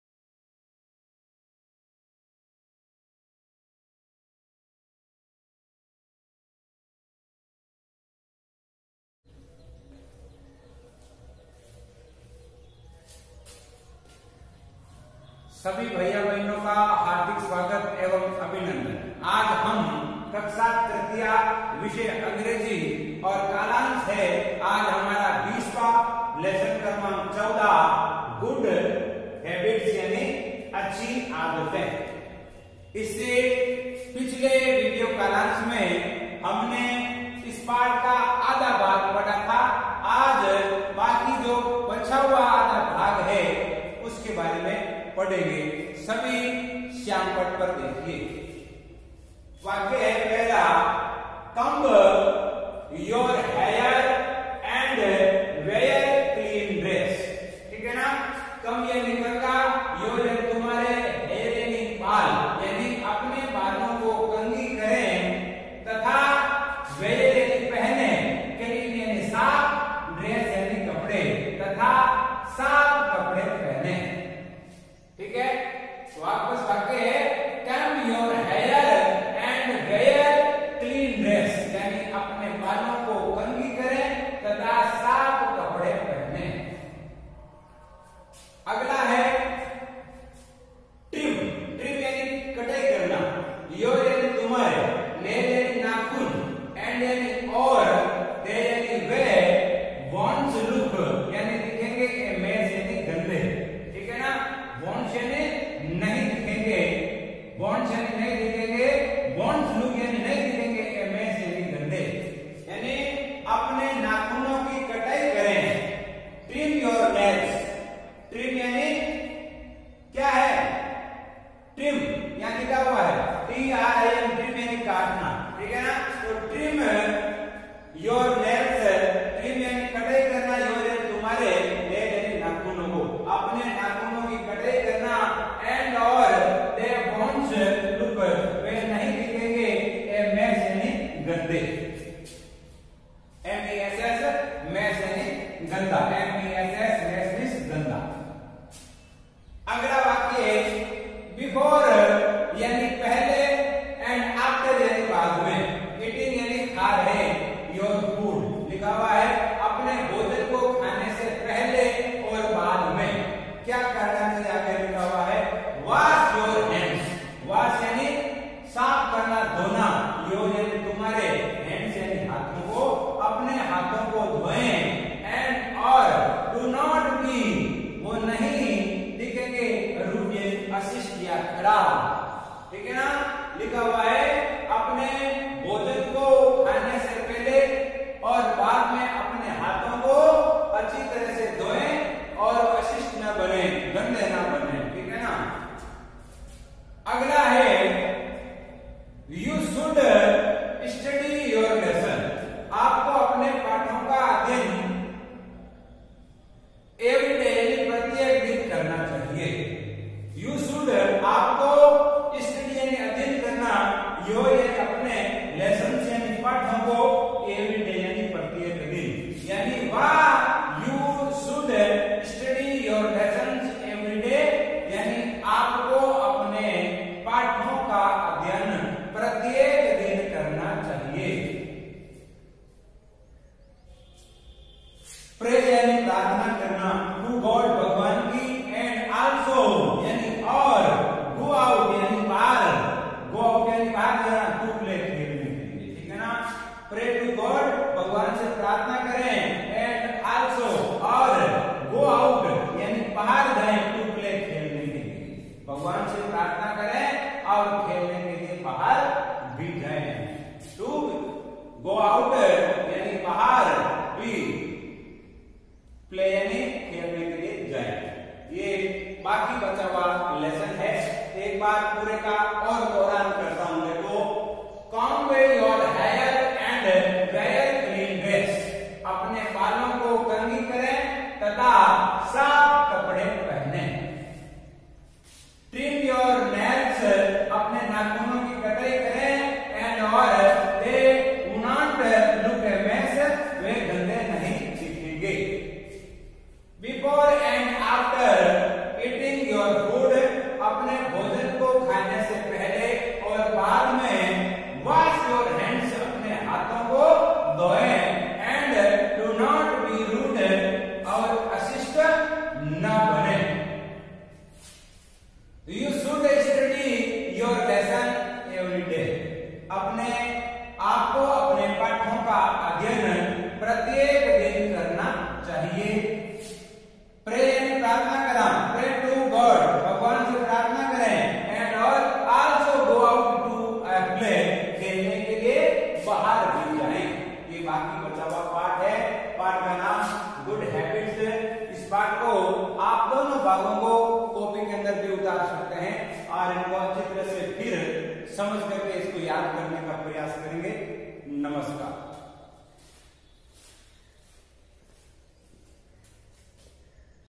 कक्षा तृतीया,विषय अंग्रेजी। कालांश 20, Lesson14, Good Habits (अच्छी आदतें)
Video from Bhanwar Lal Suthar